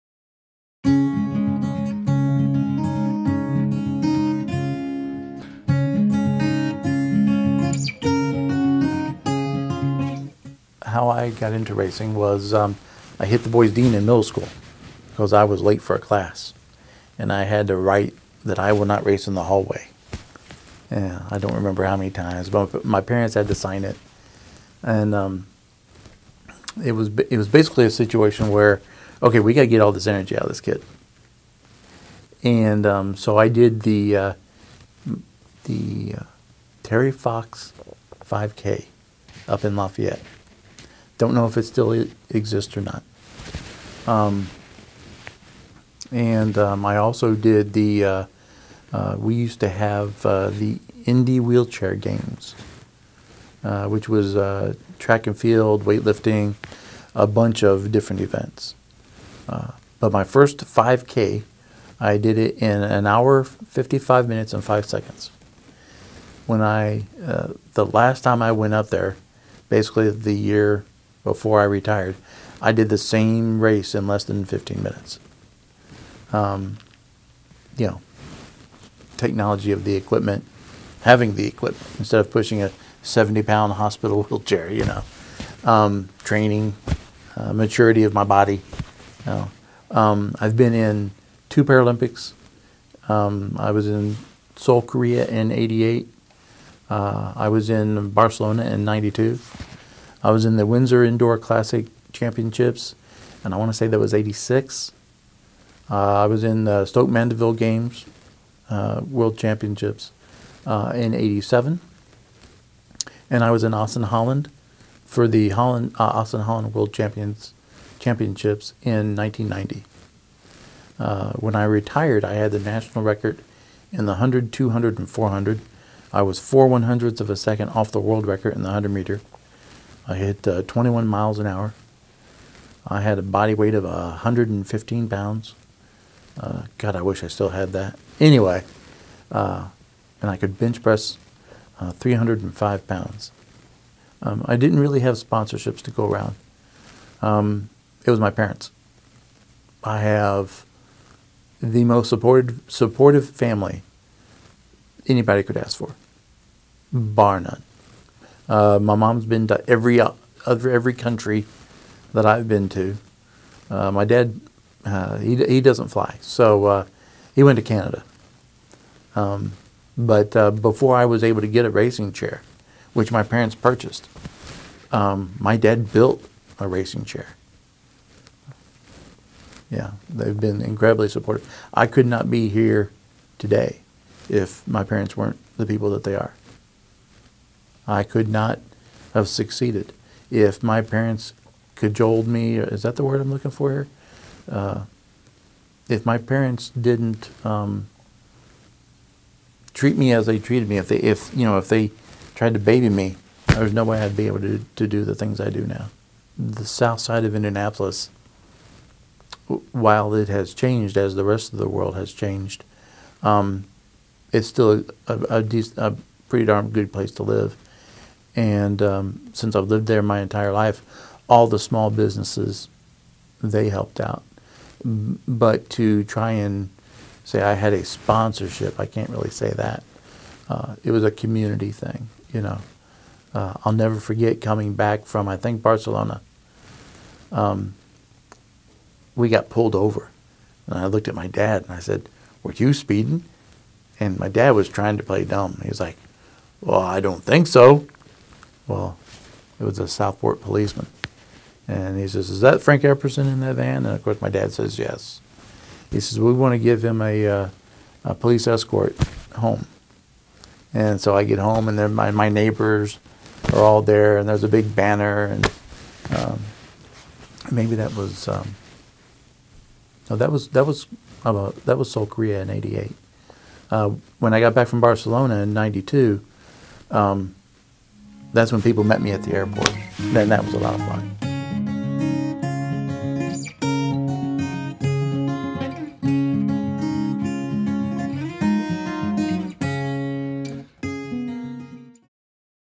How I Became a Successful Wheelchair Racing Athlete
After getting in trouble in middle school for racing the hallways, Frank's parents decided he needed an outlet for all his extra energy and helped him discover wheelchair racing. Frank went on to become a Paralympic athlete competing in two Olympics.

Video produced by Center on Aging and Community/Indiana Institute on Disability and Community/Indiana University. Funded in part by a grant from the Indiana Governor's Council for People with Disabilities